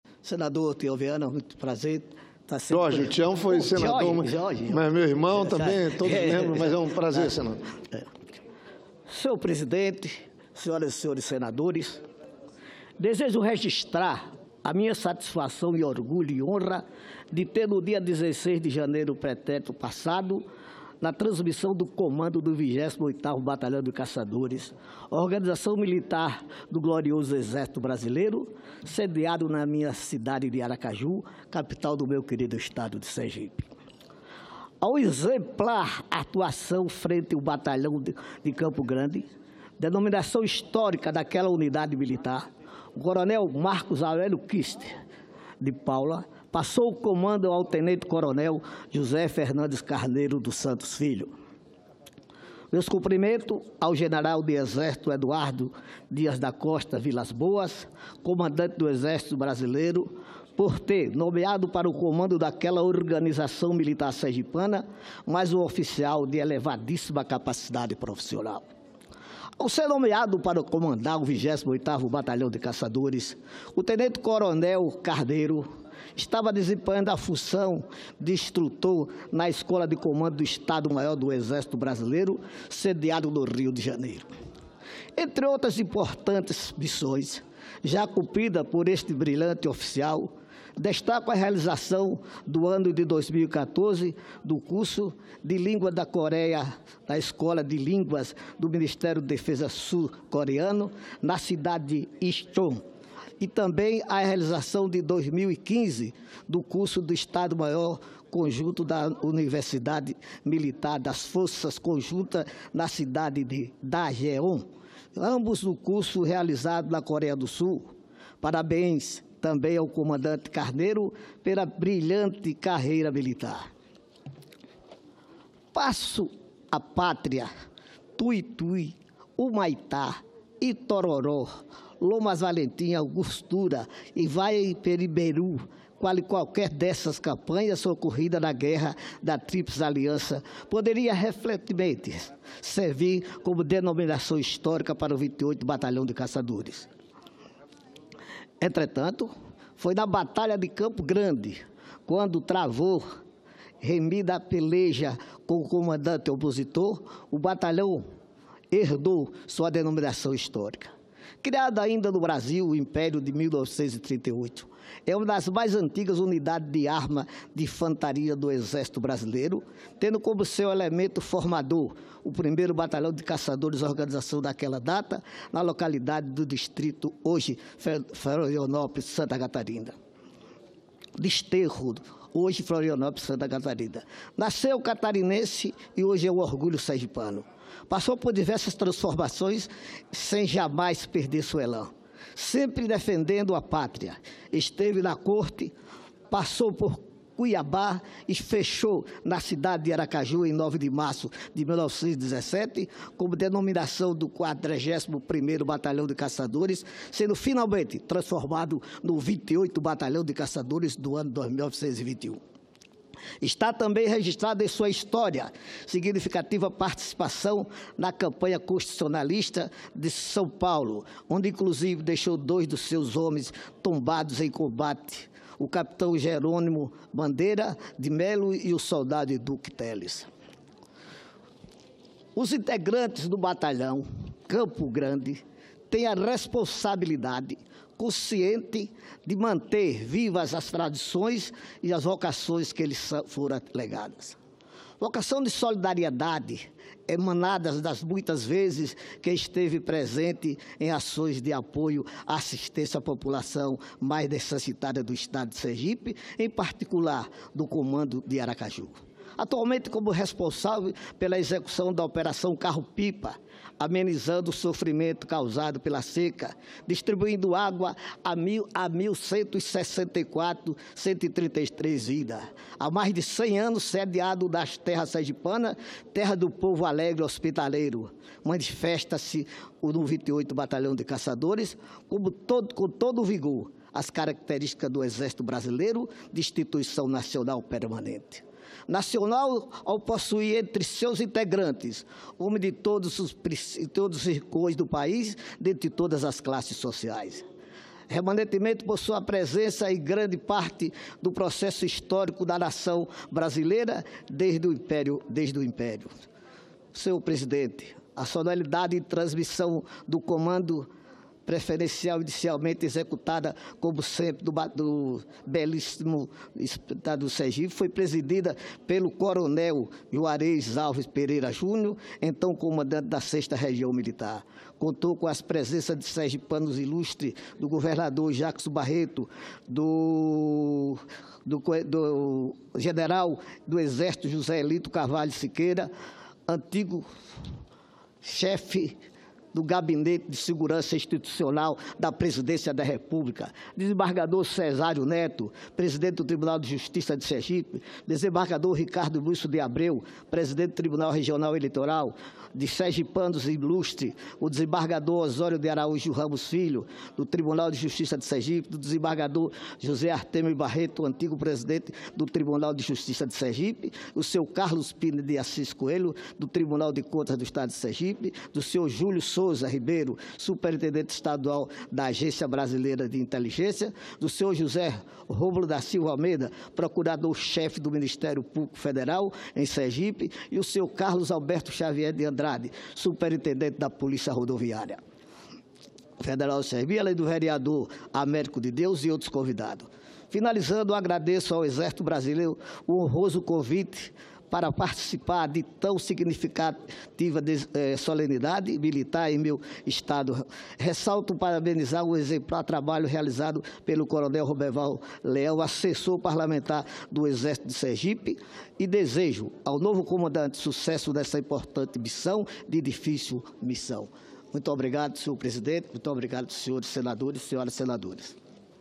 Elber Batalha relata troca de comando do Exército em Sergipe
06/02/2018 - Plenário Discursos - Elber Batalha relata troca de comando do Exército em Sergipe